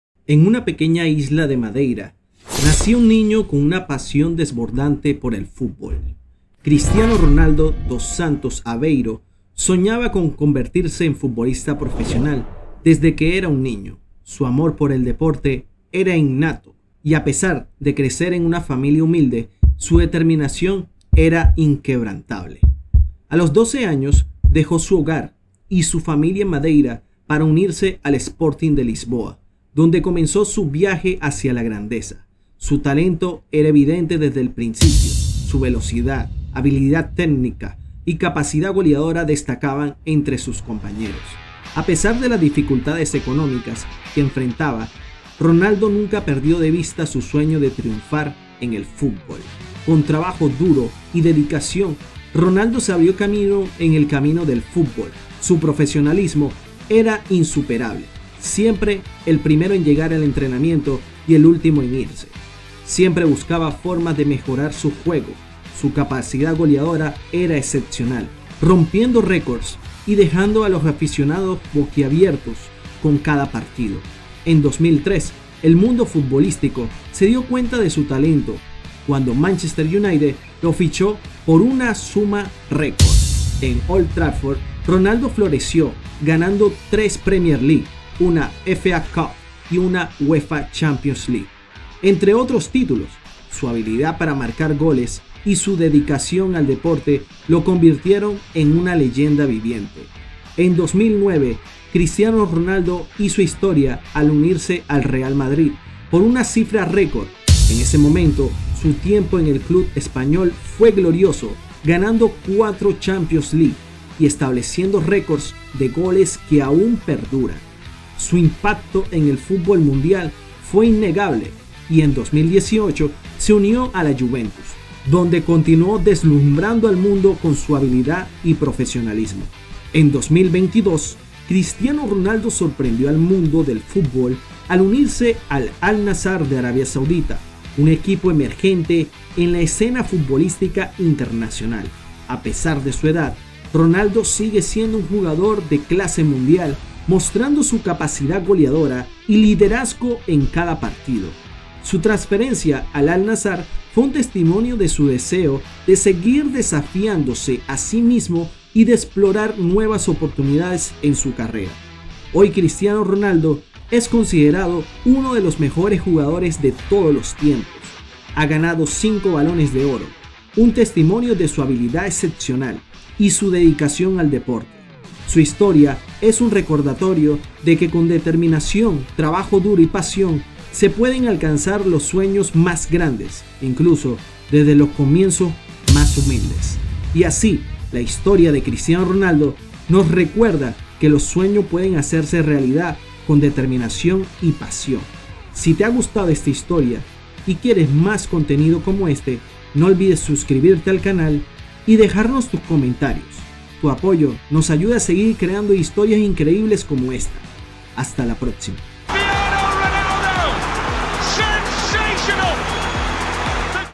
El Camino de Cristiano Ronaldo: De la Humildad a la Leyenda
🔥 ¡Bienvenidos al canal  Yo soy Ferdox! 🔥

🌟 ¡Descubre la increíble historia de Cristiano Ronaldo! Desde sus humildes comienzos en el Sporting de Lisboa, hasta convertirse en una leyenda en el Al Nasr. 🚀 ¡Sigue su viaje lleno de goles, títulos y determinación! ⚽️ ¡No te pierdas esta emocionante narrativa llena de pasión y éxito! 🏆

🌟 No te preocupes por perderte ninguna publicación, haz clic en este enlace: ➡️